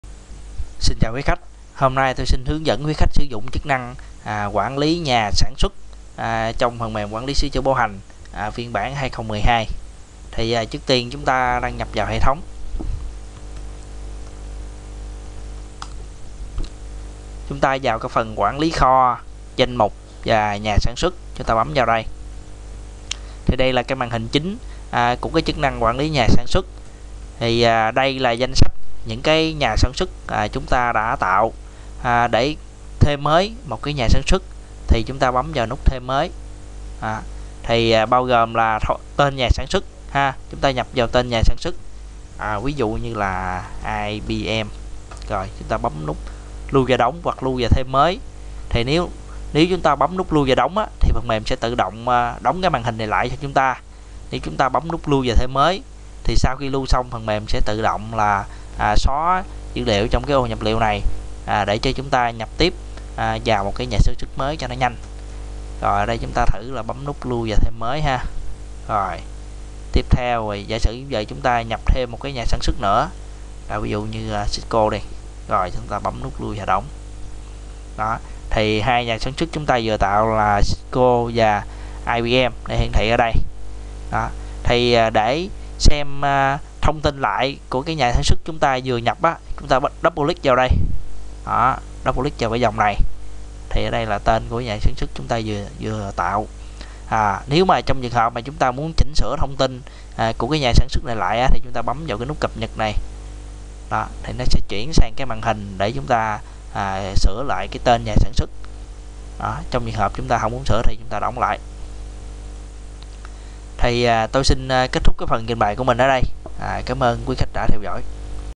Quản Lý Danh Mục Nhà Sản Xuất
Hướng dẫn sử dụng chức năng quản lý danh mục nhà sản xuất, trong phần mềm quản lý sửa chữa bảo hành 2012.